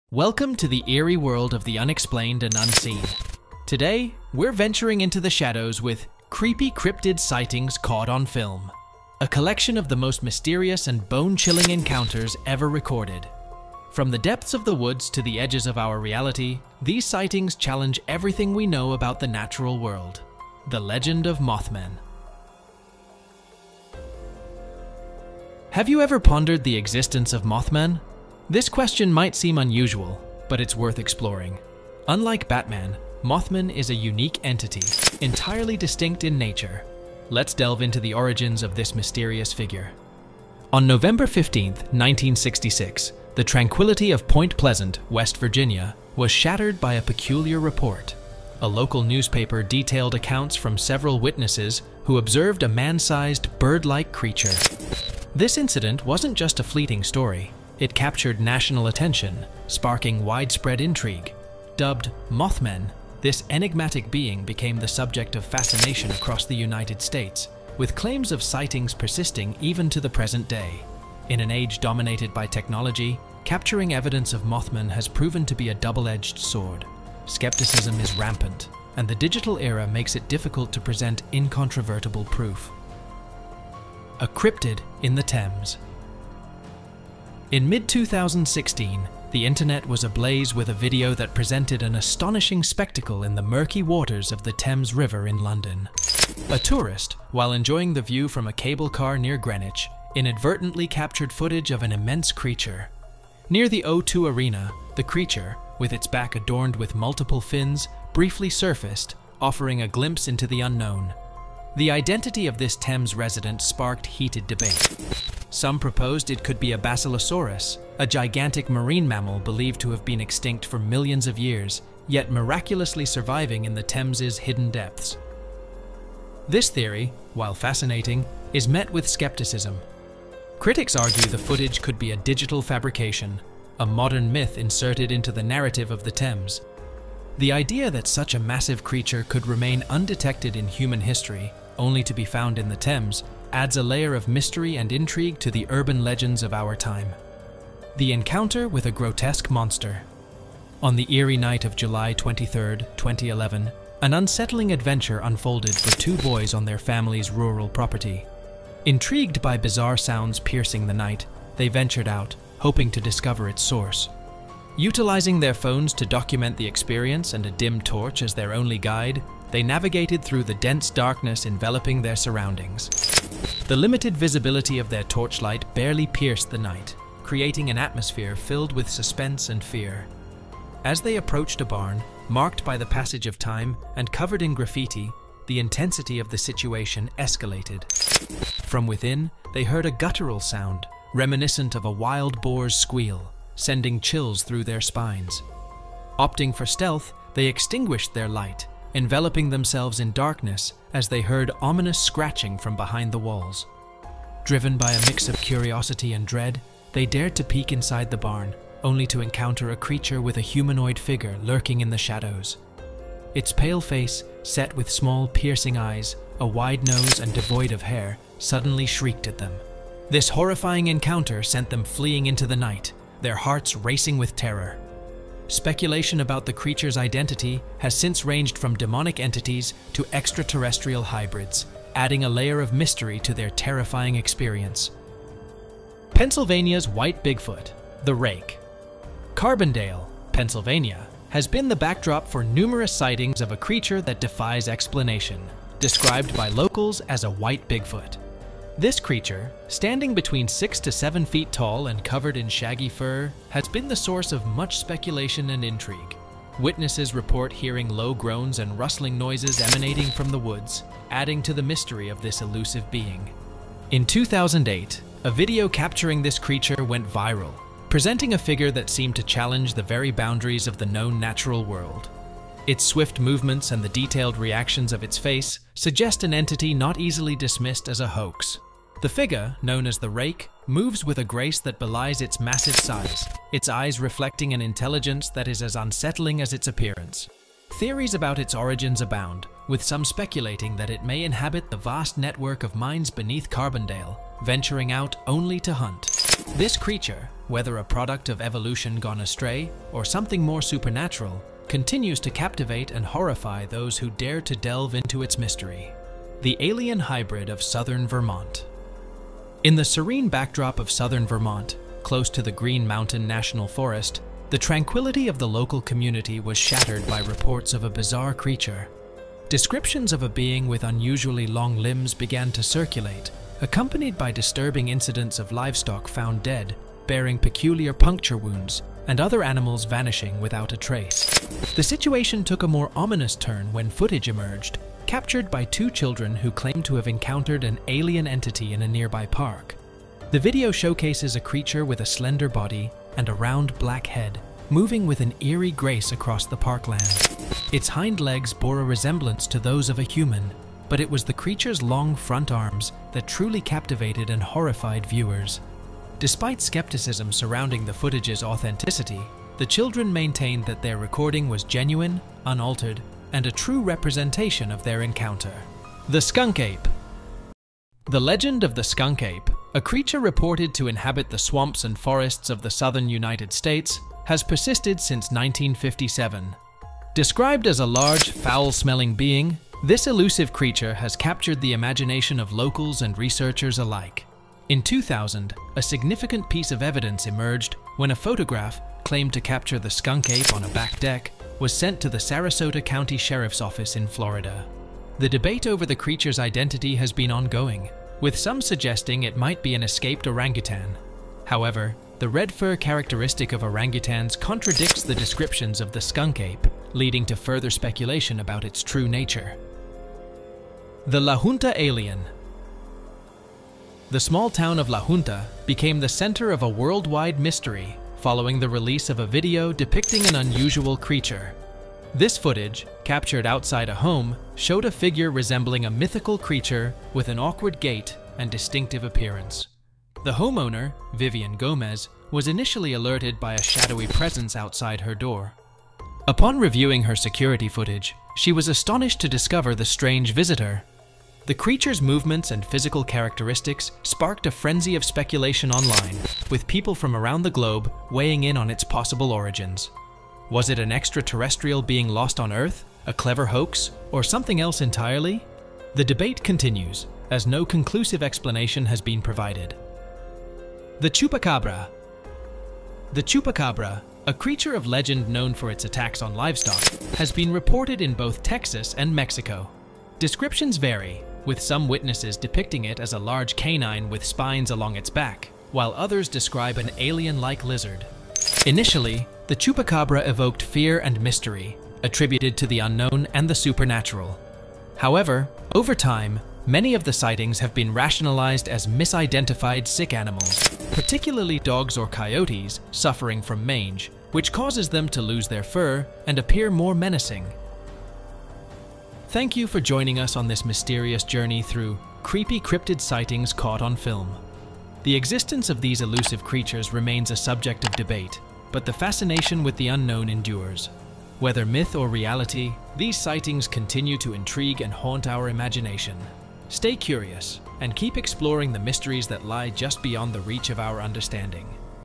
Creatures that Actually Exist
Step into the eerie world of the unexplained with our video "Creatures that Actually Exist"! Join us as we explore creepy cryptid sightings caught on film, showcasing bone-chilling encounters that challenge everything we know about the natural world. From the mysterious Mothman to other elusive creatures, get ready to be amazed.